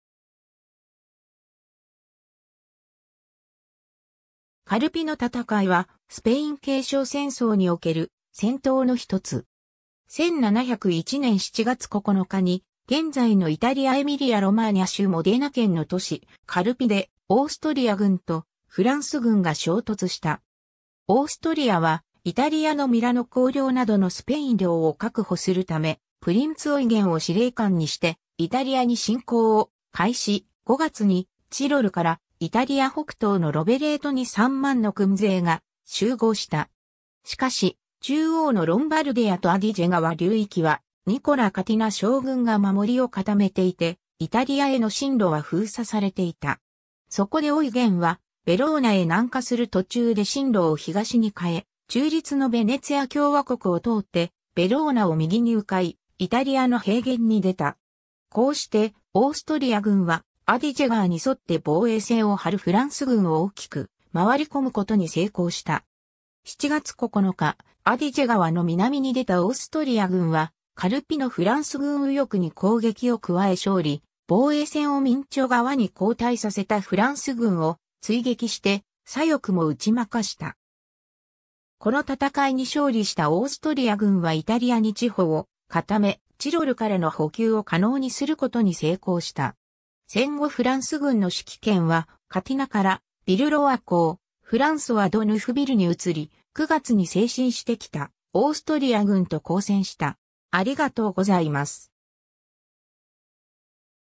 カルピの戦い
エミリア＝ロマーニャの歴史
オイゲン・フォン・ザヴォイエン
カルピの戦い（カルピのたたかい、イタリア語: Battaglia di Carpi）は、スペイン継承戦争における戦闘の1つ。
1701年7月9日に現在のイタリア・エミリア＝ロマーニャ州モデナ県の都市カルピでオーストリア（ハプスブルク帝国）軍とフランス軍が衝突した。
 オーストリアはイタリアのミラノ公領などのスペイン領を確保するため、プリンツ・オイゲンを司令官にしてイタリアに侵攻を開始、5月にチロルからイタリア北東のロヴェレートに3万の軍勢が集合した。
しかし、中央のロンバルディアとアディジェ川流域はニコラ・カティナ将軍が守りを固めていて、イタリアへの進路は封鎖されていた。
そこでオイゲンはヴェローナへ南下する途中で進路を東に変え、中立のヴェネツィア共和国を通ってヴェローナを右に迂回、イタリアの平原に出た。
こうしてオーストリア軍はアディジェ川に沿って防衛線を張るフランス軍を大きく回り込む事に成功した。
 7月9日、アディジェ川の南に出たオーストリア軍はカルピのフランス軍右翼に攻撃を加え勝利、防衛線をミンチョ川に後退させたフランス軍を追撃して左翼も打ち負かした。
この戦いに勝利したオーストリア軍はイタリアに地歩を固め、チロルからの補給を可能にする事に成功した。
戦後フランス軍の指揮権はカティナからヴィルロワ公フランソワ・ド・ヌフヴィルに移り、9月に西進して来たオーストリア軍と交戦した（キアーリの戦い）。